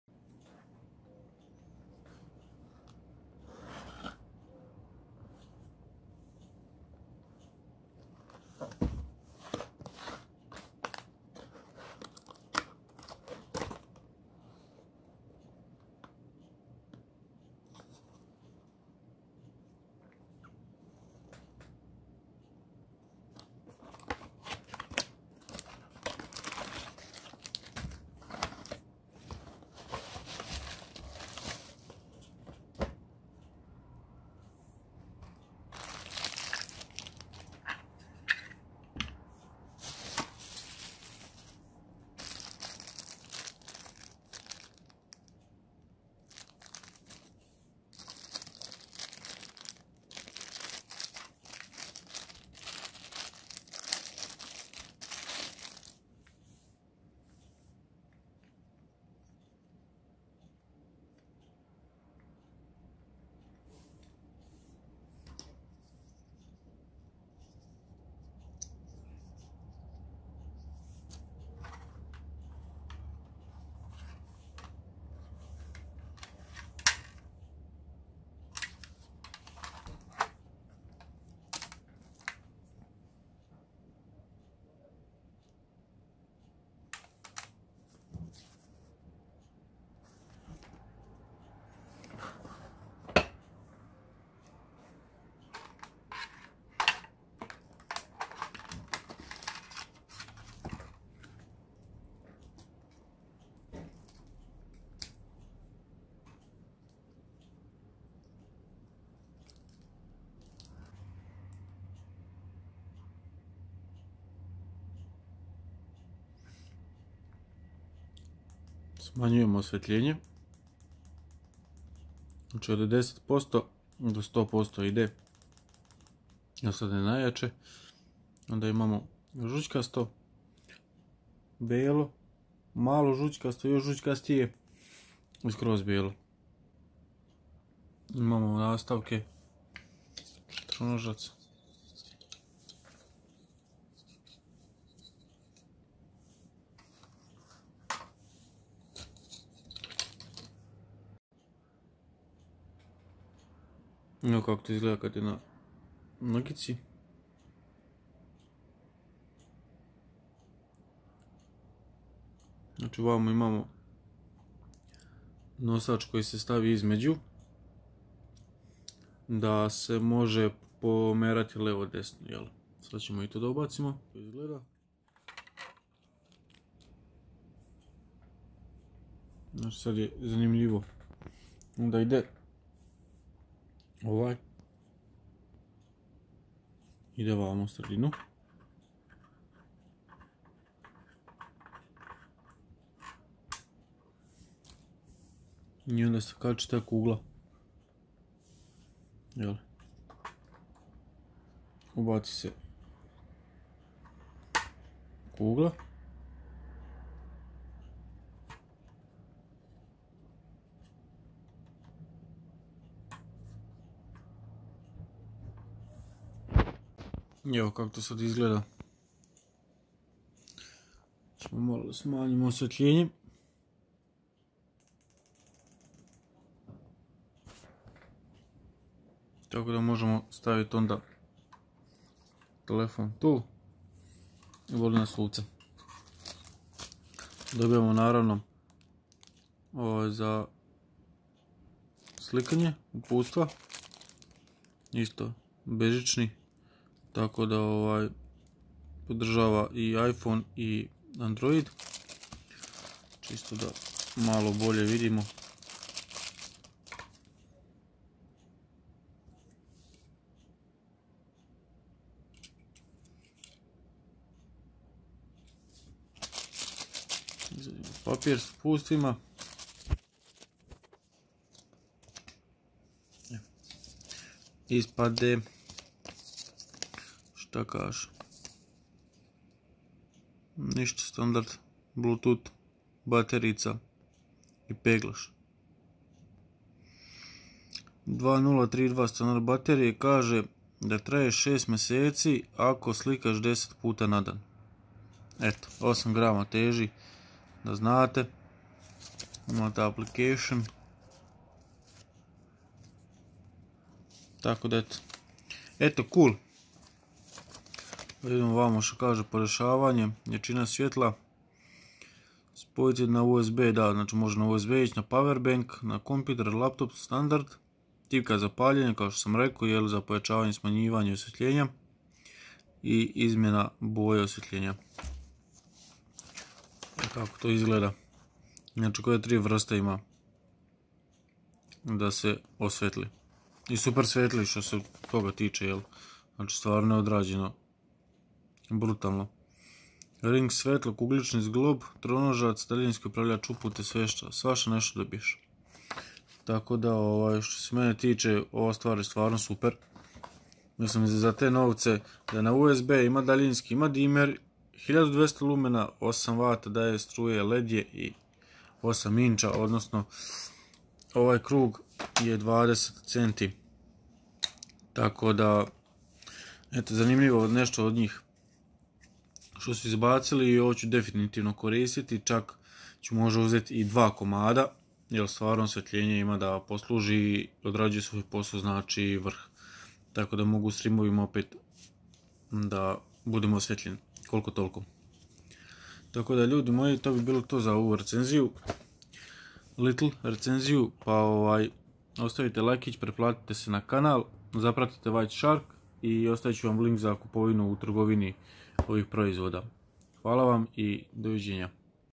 WHITE SHARK MOONLIGHT | RING LED LIGHT 20 CM | RL-08
⚠️ KUPITE U VACOM POSLOVNICAMA ILI PREKO VACOM WEB SHOPA !!! ⚠️

❤️ DETALJI:
▶--  Vodootpornost: Ne
▶--  IC daljinski upravljač: Da
▶--  Napajanje : USB 5V
▶--  Usmjerenost: Omnidirekcijski
▶--  Snaga (W): 8
▶--  Lumeni: 1200
▶--  Prigušavanje: 10% - 100%
▶--  Promjer vanjskog kruga (cm): 20
▶--  Promjer LED kuga ("): 8

Ukljucite  🔔 i nikada nećete propustiti moj video !